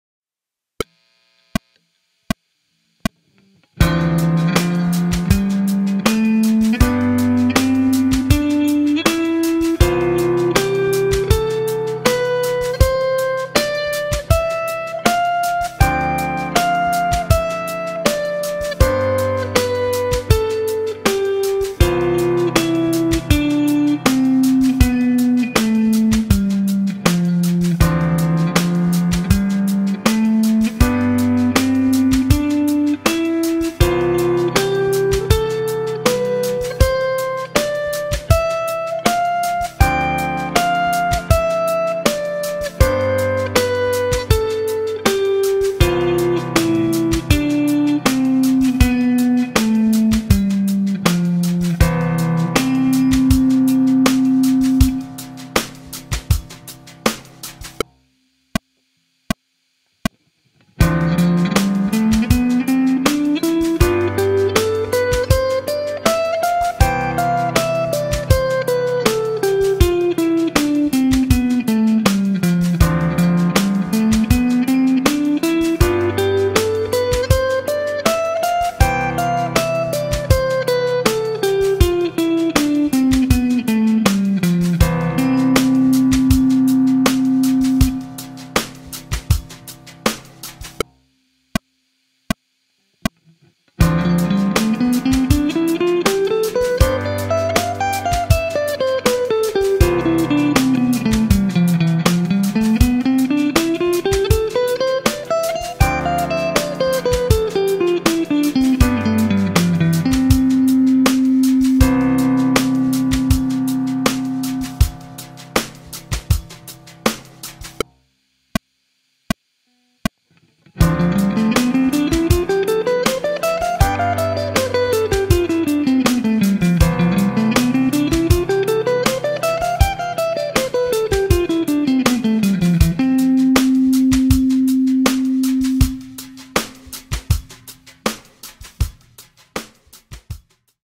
Escala Maior - CAGED - Shape de C
Esse é o primeiro video da matéria de escalas do módulo 3 do Pega a Guitarra.

Vamos começar com a escala maior no shape de C.

Temos a tônica da escala na quinta corda, e a sua estrutura toda para trás, assim como o acorde formado pelo shape de C. (Caso não tenha entendido nada, procure o video Harmonia 1)

Apesar da tônica estar na quinta corda, começaremos pela sexta corda porque vamos aprender todas as possibilidades de notas nessa região.

Use a ferramenta de velocidade do youtube para aumentar ou diminuir a dificuldade do exercício.

Bons estudos!